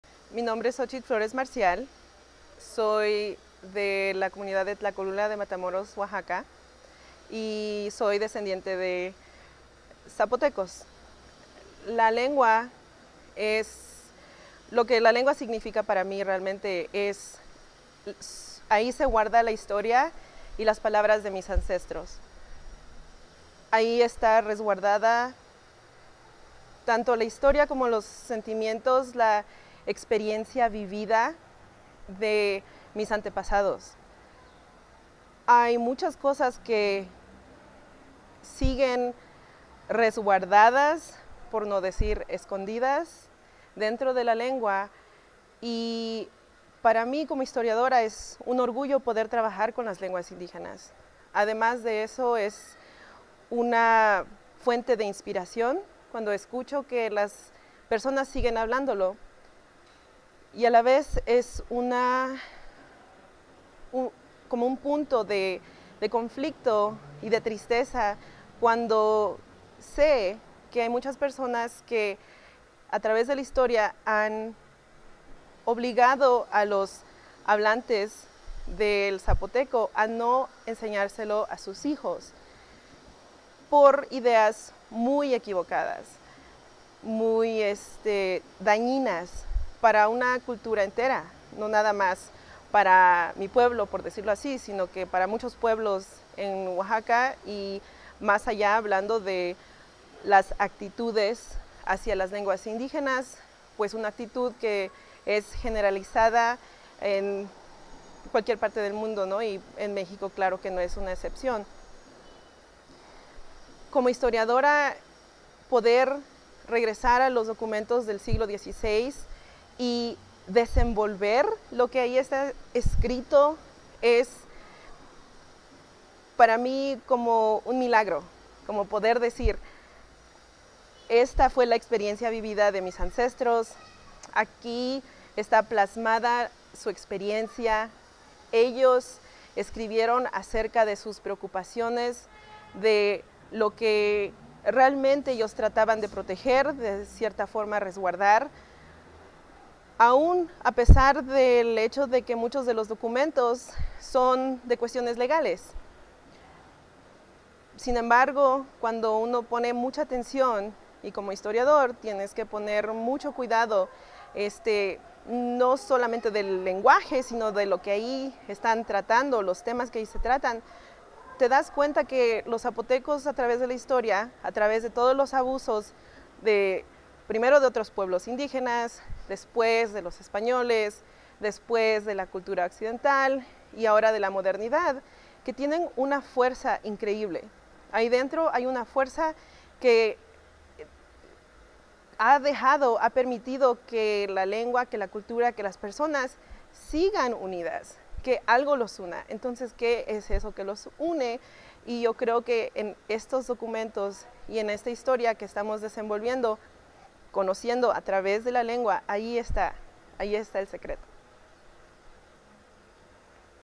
Xochitl Marina Flores Marcial sobre la lengua zapoteca / on the Zapotec language
La historiadora Xochitl Marina Flores Marical habla sobre la lengua zapoteca y la significa que tiene para ella y su comunidad.

En español.

This interview was created in June 2015 in Oaxaca City, Oaxaca, Mexico as part of the 2015 Linguistics Field School, funded by NSF REU Site Grant #1461056 (PI K. David Harrison, Co-PI Brook Danielle Lillehaugen & Ted Fernald).